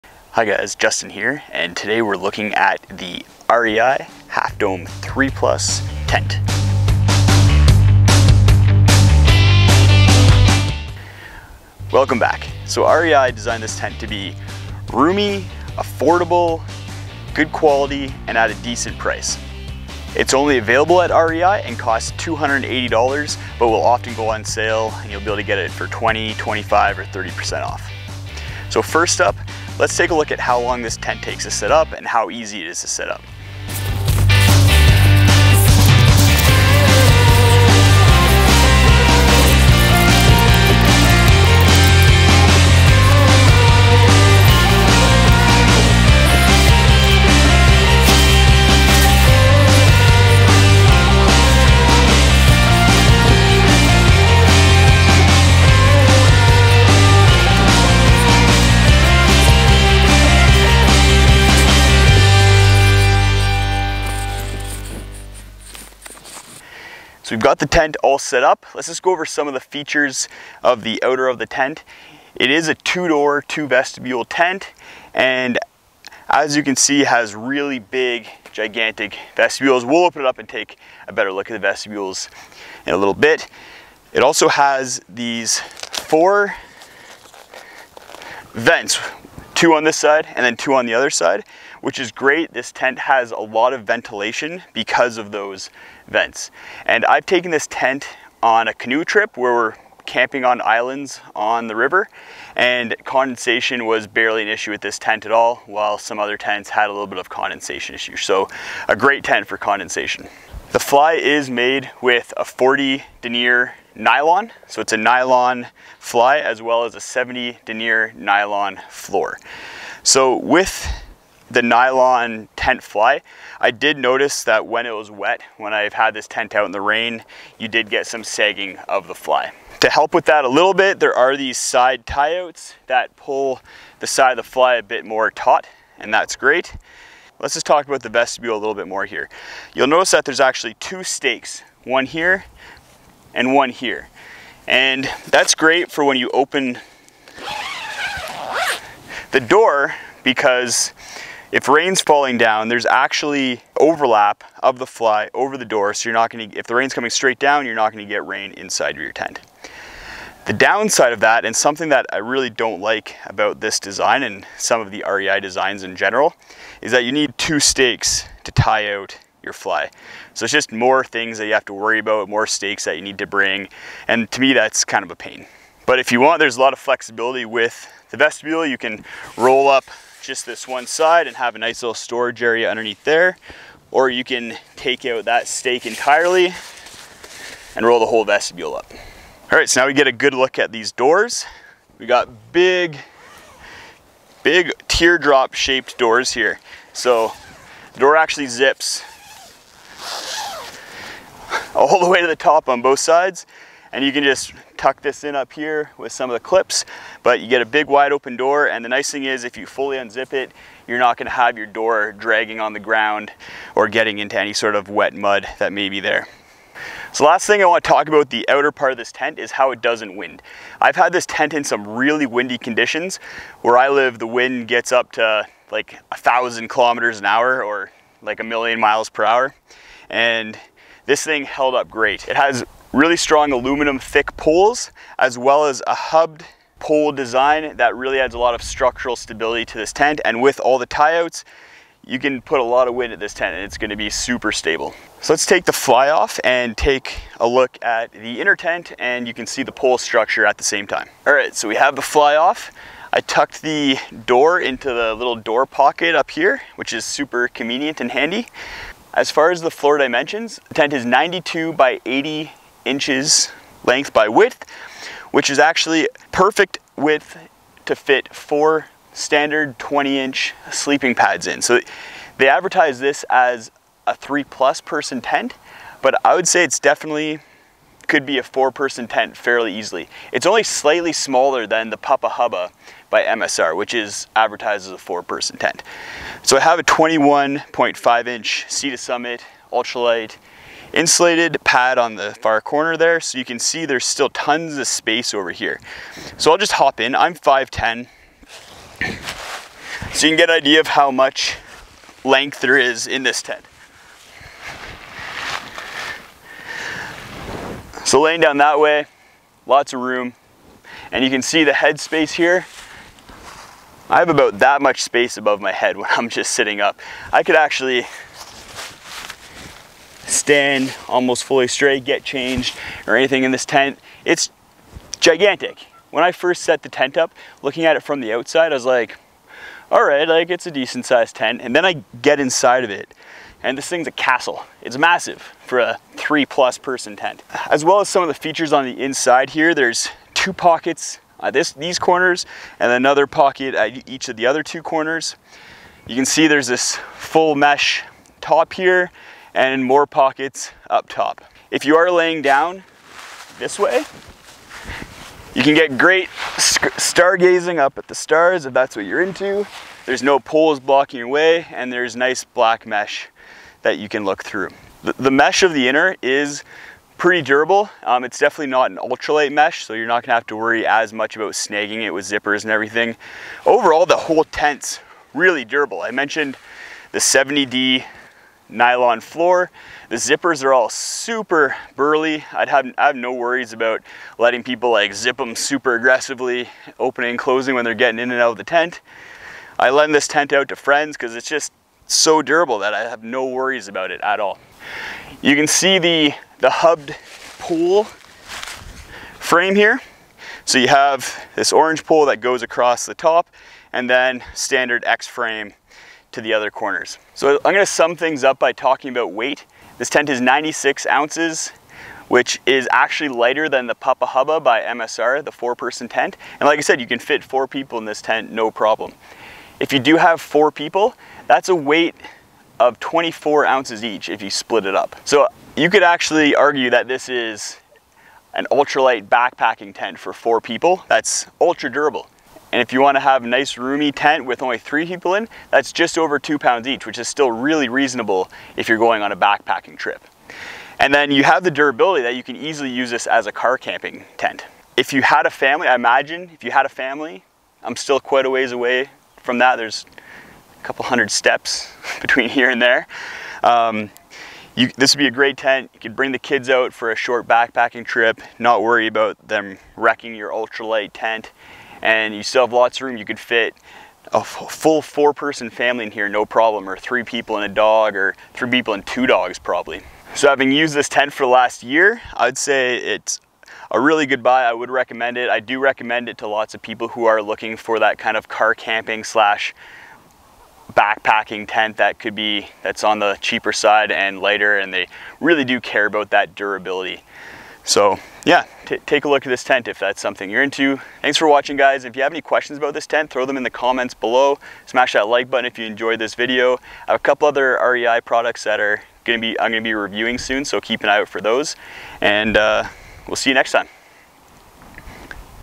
REI Half Dome 3 Plus - Ultralight & Durable Group Tent
In this video, I review the REI Half Dome 3+ tent that was released in 2018. I go over the specs, features, and downsides of the tent. In the end, I give an overview of how I think this tent should be used and who it would work well for.

- MY FAVORITE GEAR -
Garmin InReach Mini
Trekology 2.0 Pillow
REI Flash 55 Pack
Enlightened Equipment Enigma Quilt
Garmin Fenix Watch
Lanshan 1 Tent